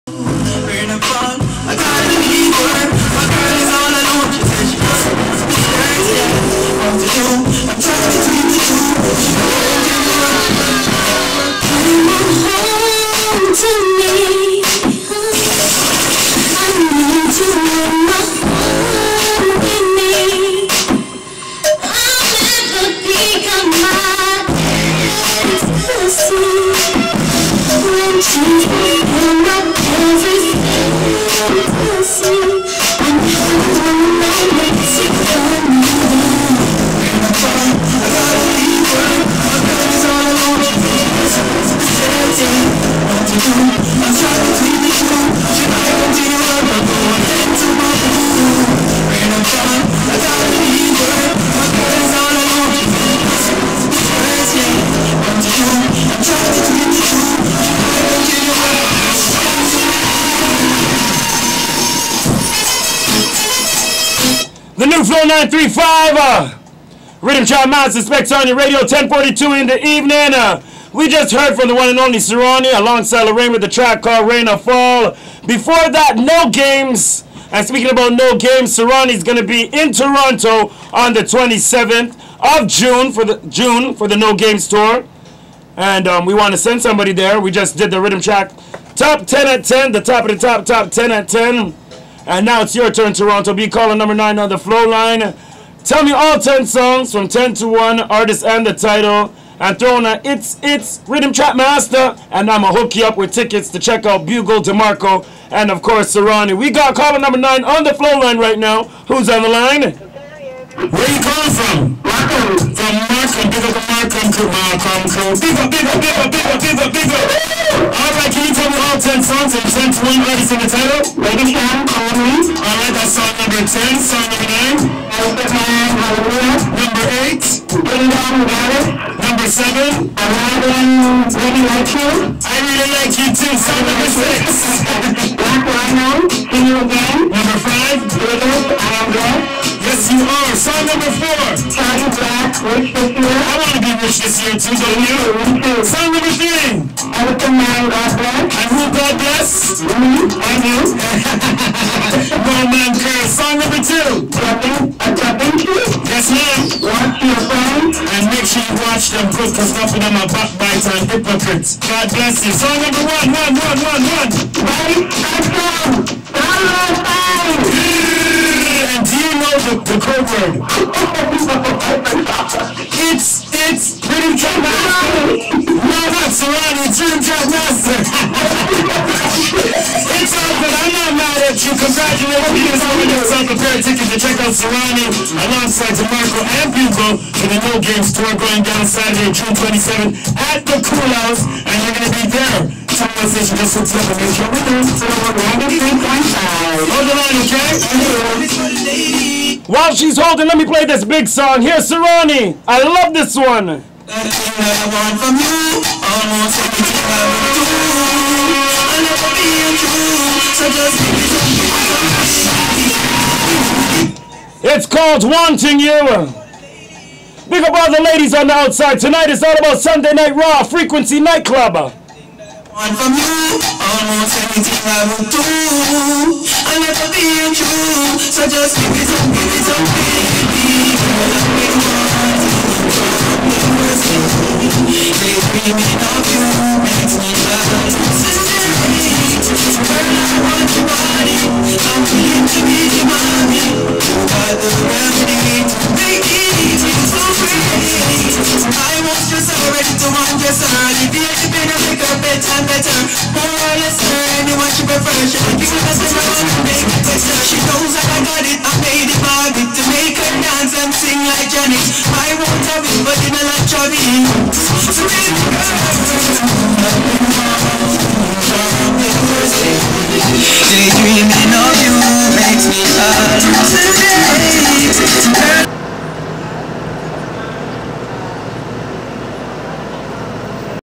SPEX @ Flow 93.5 May 29 (p2)
Spex spinning at the Flow 93.5 studio, Toronto. May 29, 2009. Hight Quality Video